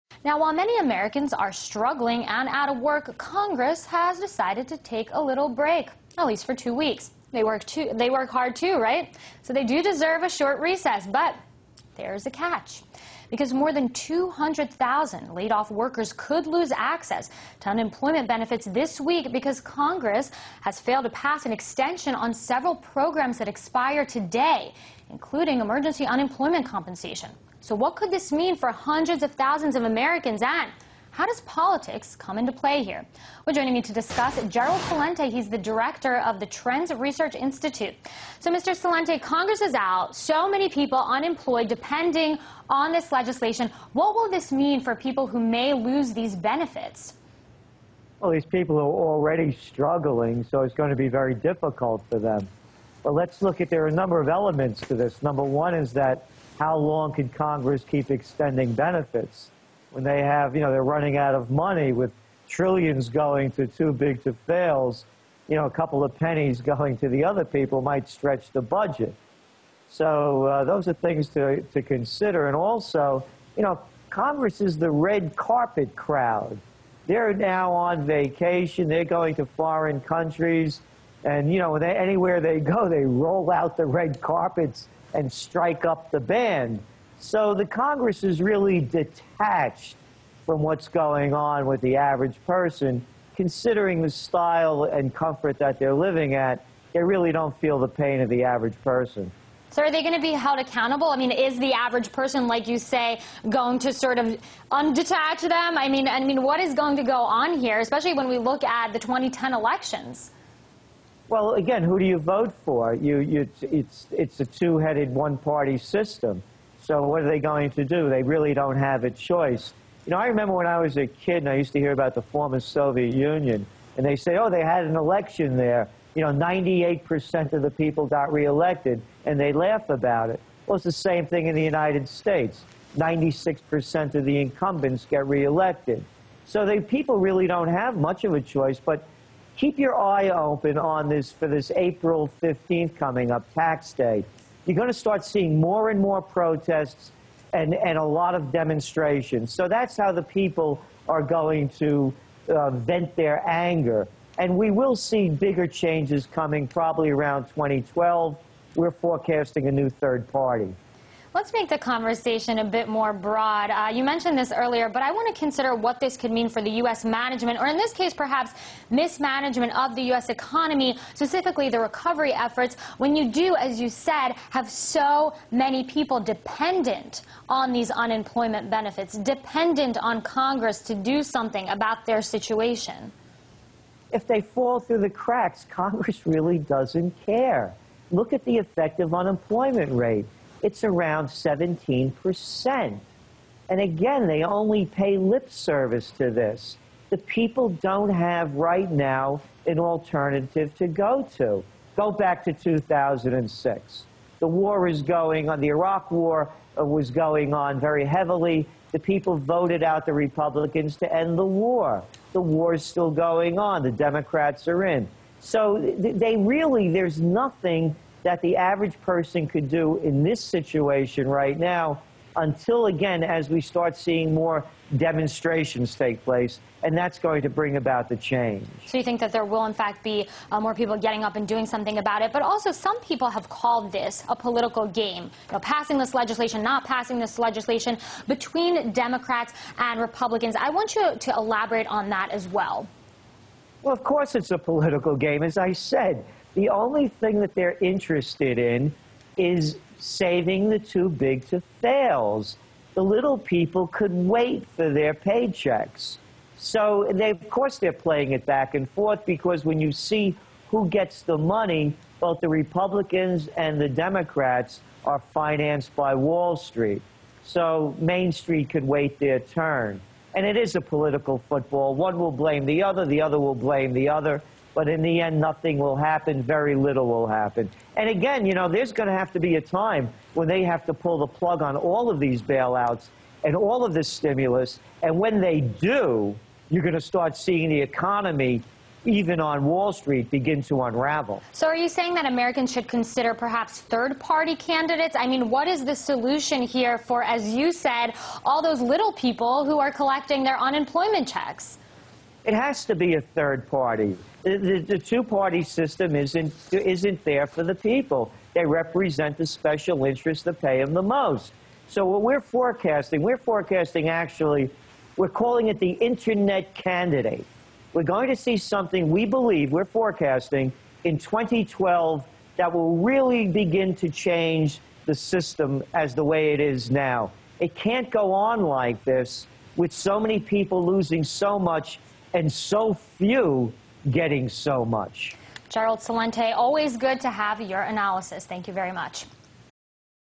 Gerald Celente: Forecast for Third Party "Internet Candidate" for 2012
RTAmerica — April 05, 2010 — More than 200,000 people will lose their unemployment benefits today. Congress has gone on break for a recess but is this the right time? Gerald Celente says that congress is the red carpet cloud, they are going on vacation and they are detached from what the average person is feeling; they dont feel the same pain that the economy has put on people.